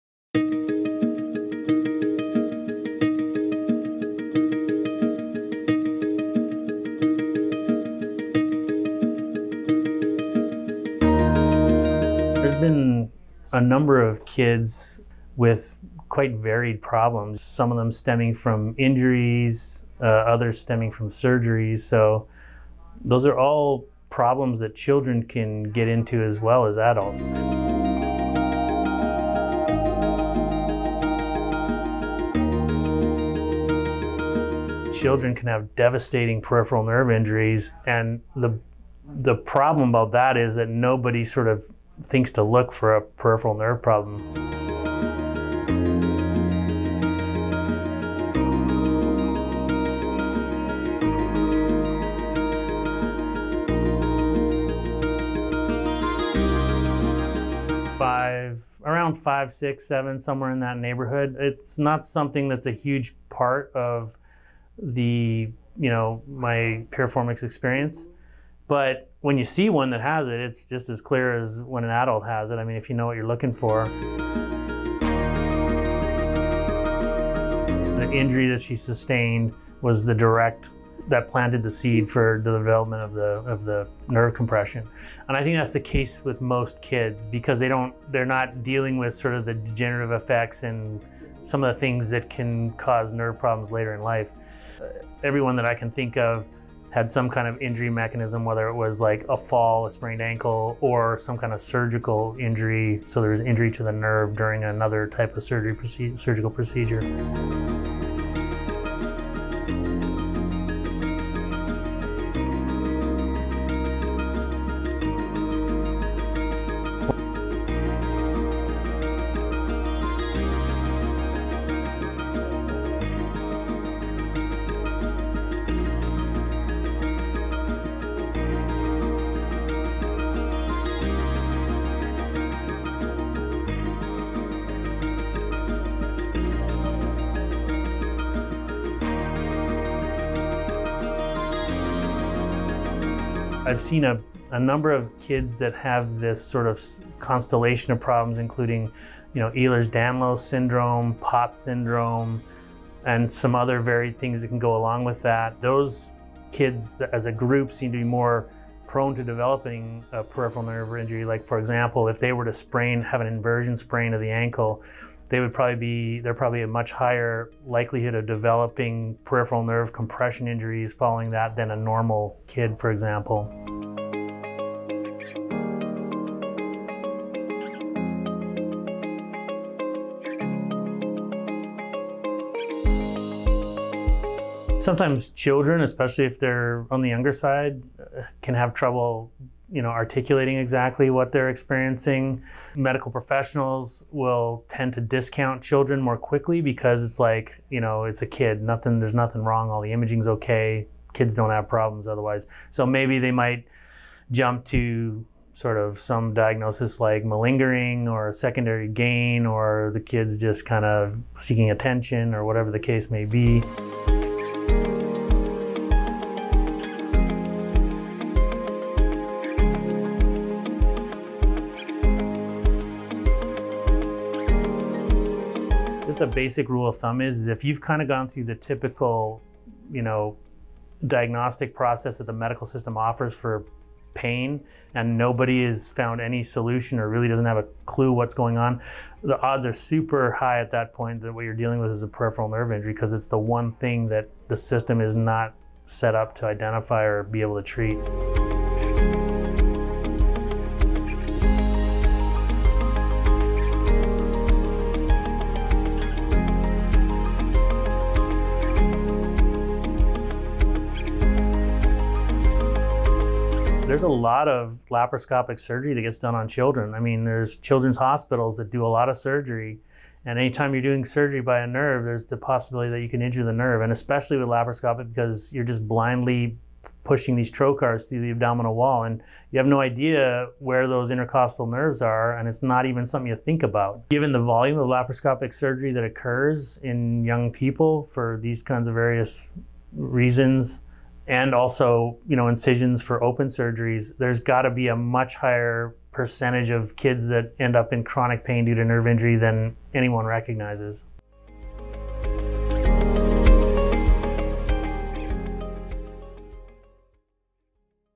Peripheral Nerve Damage in Kids and Teens
Peripheral nerve injuries in kids often go undiagnosed which can lead to prolonged pain and suffering for many children and teens.
As Dr. Tollestrup explains, most in the medical community are not knowledgeable about peripheral nerves and would not consider looking for this problem in younger people.
But the rule of thumb for peripheral nerve damage remains the same. If the patient has searched for answers, can't find any and the imaging is clear, most likely there is a peripheral nerve injury.